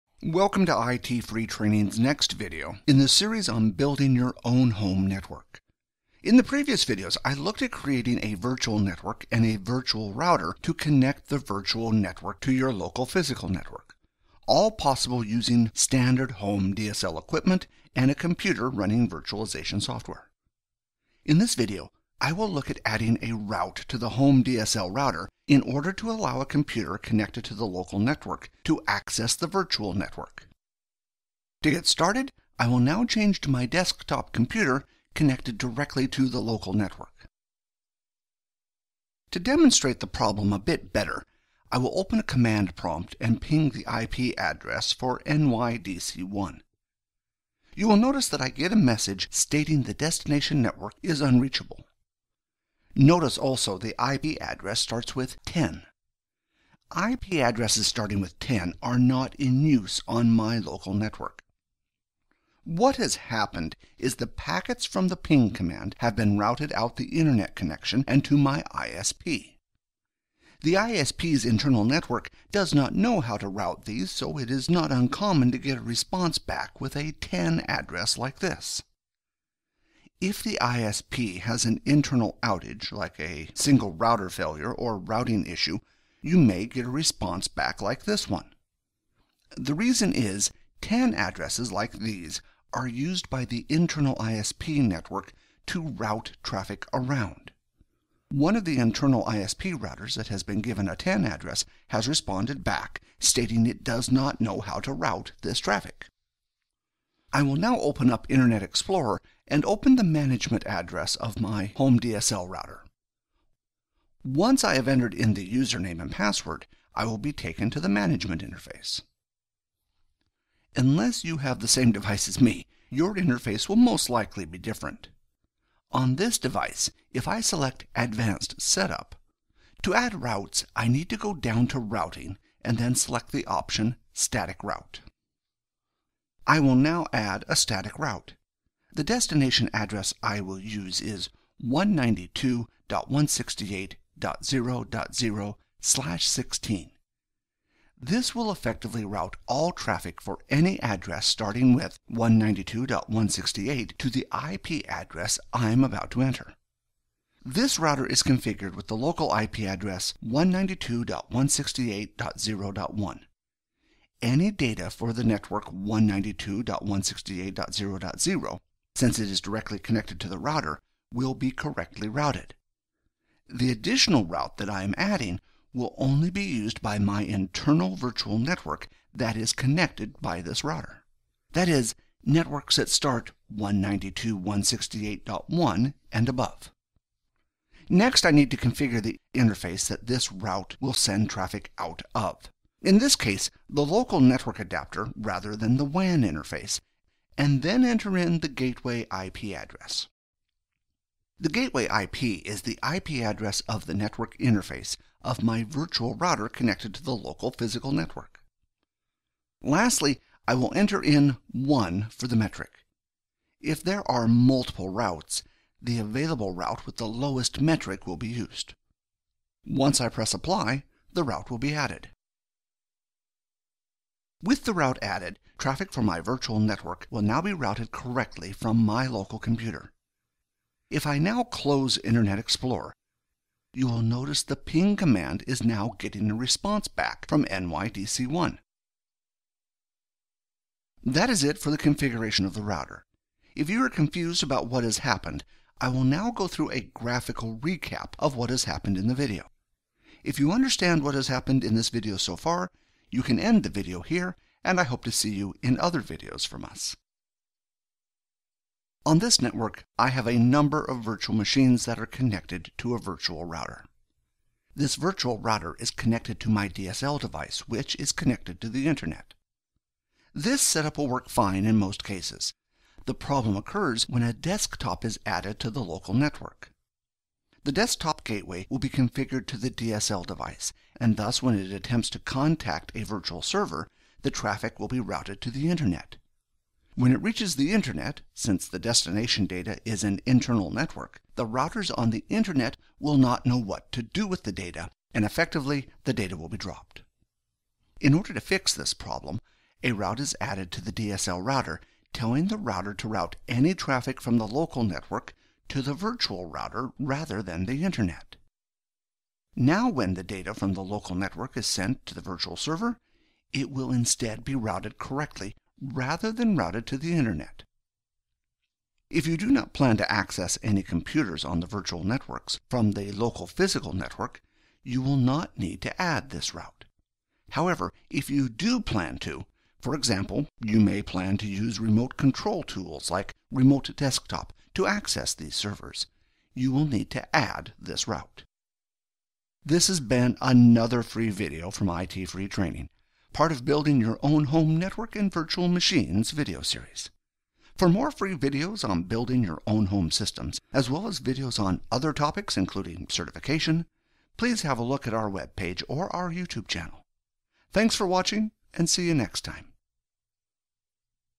Home Lab DSL Configuring
This video looks at how to add a route on your local DSL device to route traffic from a local network to a virtual router created in a previous video. If this route is not present, computers on the local network will not be able to contact virtual computers on the virtual network.

Demonstration
By default, computers on your local network will route traffic to your local DSL device. The DSL device will route this traffic to your ISP which will not know where to send it. When looking at responses back from the network, you may get unusual results. For example you may get a result back from an address starting from 10. This occurs because an internal device in your ISP is responding back stating it does not know how to route that traffic.

In order to add a route to your home router you need to access the admin interface of the router. If you run the command IPConfig on your local computer, the gateway address given will usually be the address of the management interface for that device. In this case, the IP Address is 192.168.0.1. Different devices have different interfaces. Essentially you need to find an interface in which you can add routes. The route to be added is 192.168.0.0/16. All the virtual machines use an address that is starting with 192.168. In this case, the route will have the gateway address of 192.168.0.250. This is the IP Address of the virtual router on this network.

Recap
Shown here is the virtual router which is connected to the local area network and also the virtual computers. This works well until you attempt to contact a virtual machine from a desktop computer that is connected to the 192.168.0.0 network. This traffic will be routed to the Internet. In order to have this corrected, a route is added to the DSL device which will route any traffic for the internal virtual network to the virtual router. This will allow computers on the local area network to access virtual networks without having to change any configurations on the local computer.

for our always free training videos. This is only one video from the many free courses available on YouTube.

References
None